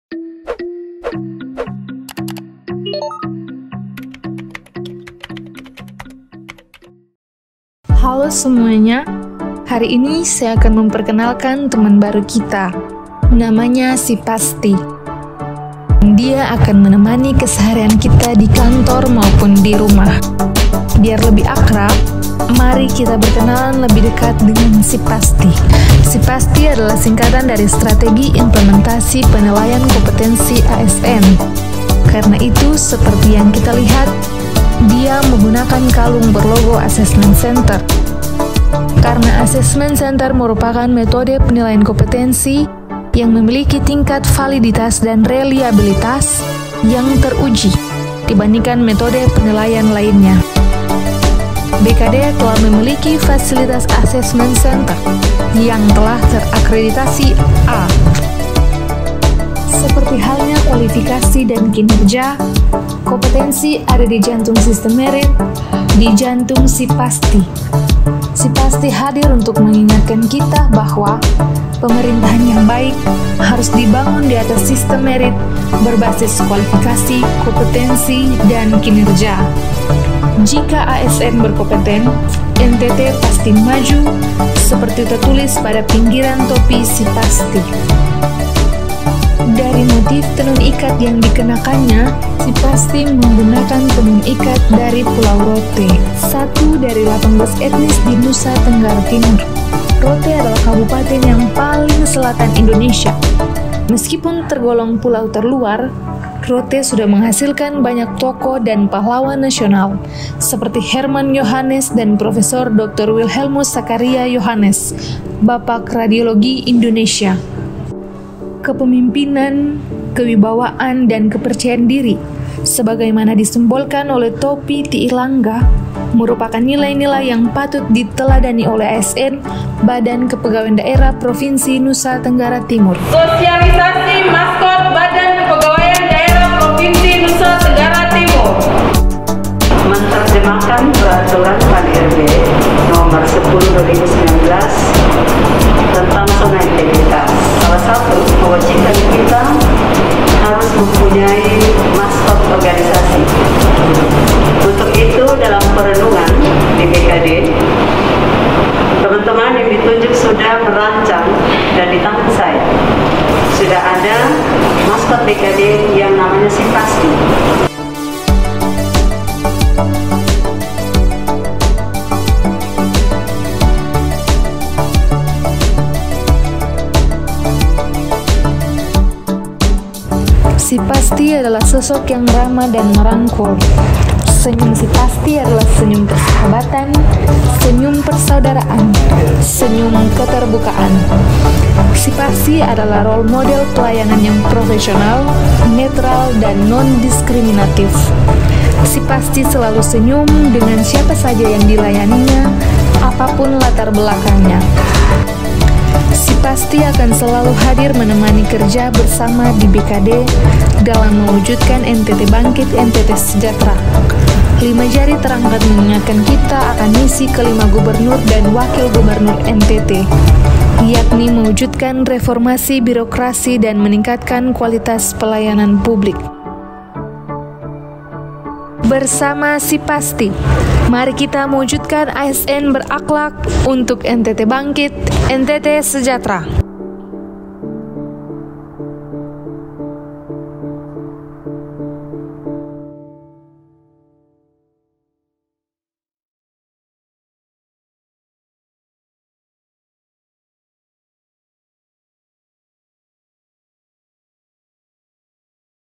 Yuk, Kenalan Dengan "SI PASTI" Maskot BKD Provinsi NTT! | Badan Kepegawaian Daerah Provinsi NTT
Yuk kenalan dengan SI PASTI, maskot Badan Kepegawaian Daerah Provinsi NTT. SI PASTI hadir dan akan terus menemani kerja di Badan Kepegawaian Daerah Provinsi NTT.

Creator Maskot Si Pasti :
    Niko Muni

Cameraman/Videographer :
    Kevin Daud

Voice Over :
    Cynthia D. Pattiasina

Animator  + Editor :
    R.A.Welvart